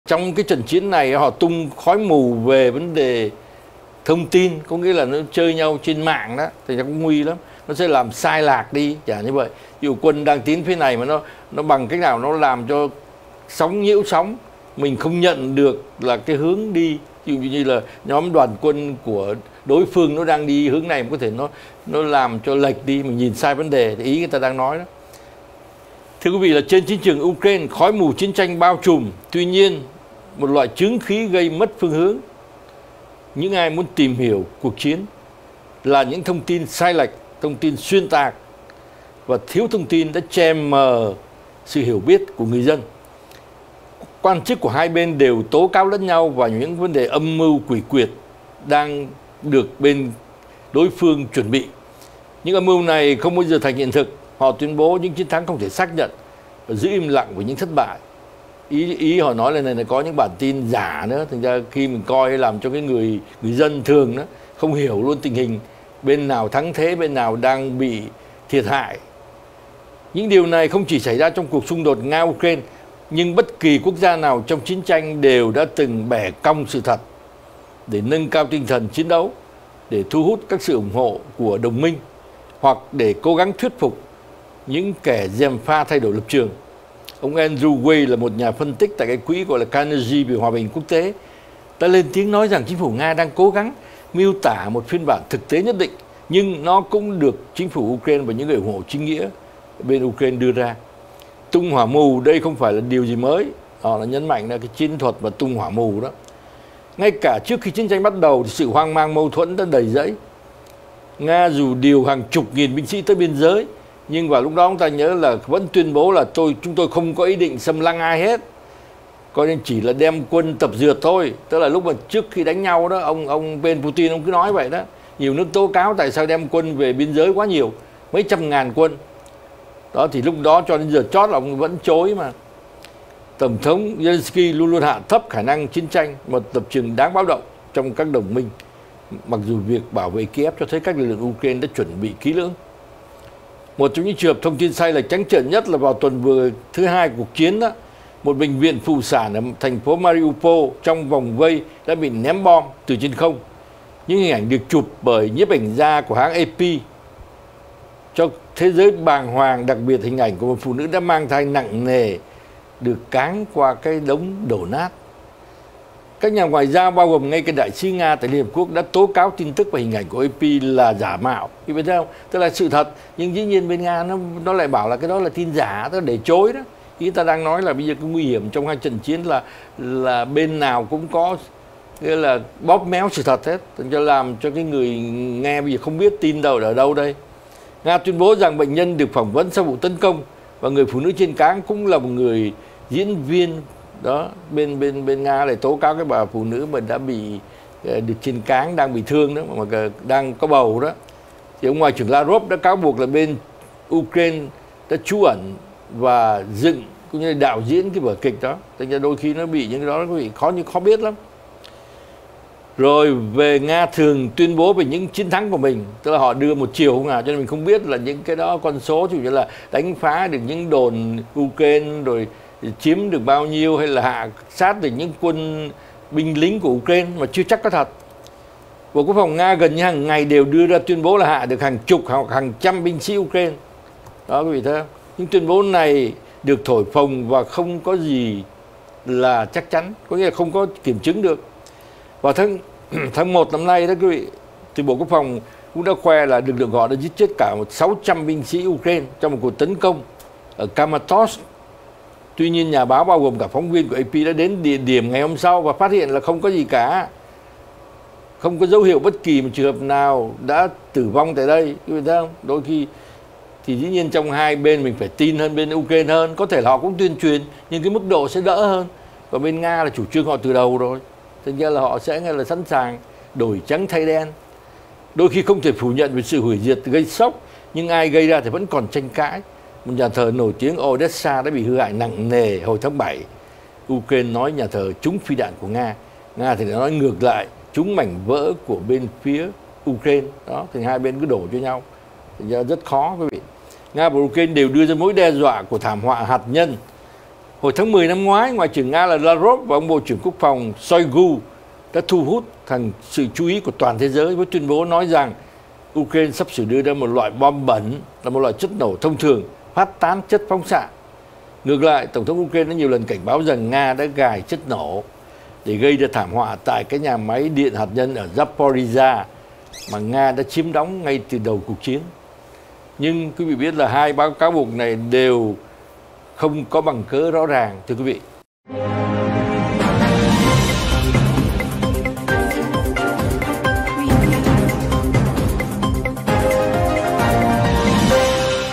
Chiến tranh Ukraine và cuộc chiến ‘tung hỏa mù’ thông tin
Trên chiến trường Ukraine, khói mù chiến tranh bao trùm binh lính. Xa chiến trường, một loại chướng khí liên quan gây mất phương hướng làm ảnh hưởng đến những ai muốn tìm hiểu những gì đang diễn ra trong cuộc chiến.
Thông tin sai lệch, thông tin xuyên tạc và thiếu thông tin đều che mờ sự hiểu biết của người dân. Các quan chức của mỗi bên tố cáo những âm mưu quỷ quyệt đang được kẻ thù chuẩn bị, những âm mưu này không bao giờ thành hiện thực. Họ tuyên bố những chiến thắng không thể xác nhận — và giữ im lặng về những thất bại.

Quý vị cũng có thể xem trực tiếp trên:
Channel 55.12 (Orlando. Florida)
Roku App: Vietmedia Channel